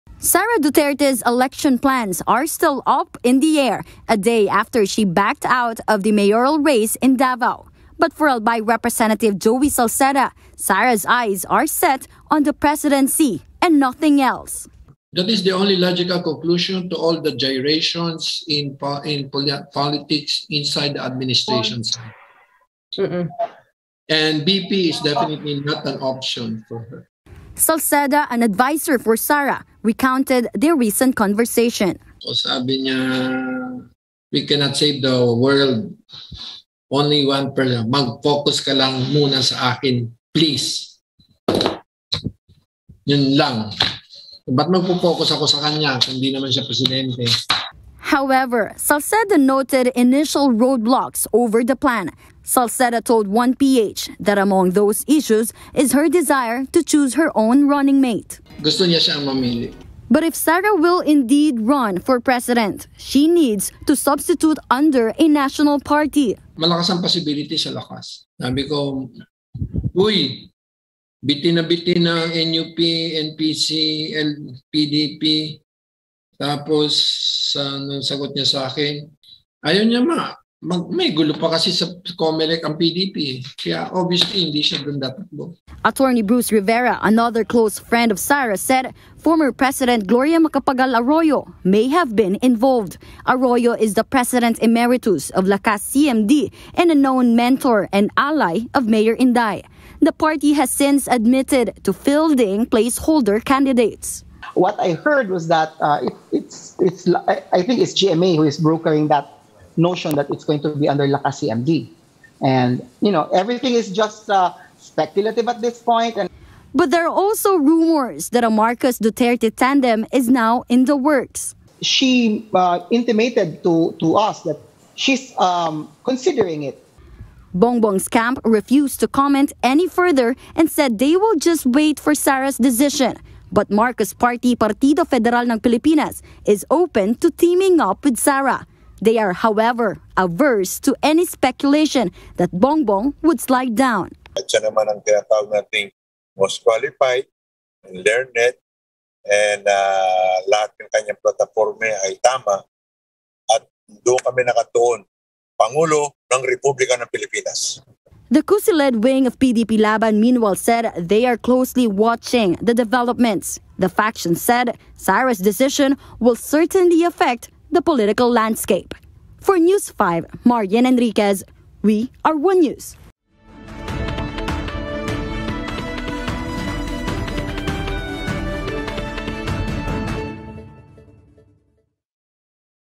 Sara Duterte has eyes set for presidency — Rep. Joey Salceda
Albay Rep. Joey Salceda believes Mayor Sara Duterte is eyeing to run for president in the 2022 elections and nothing else.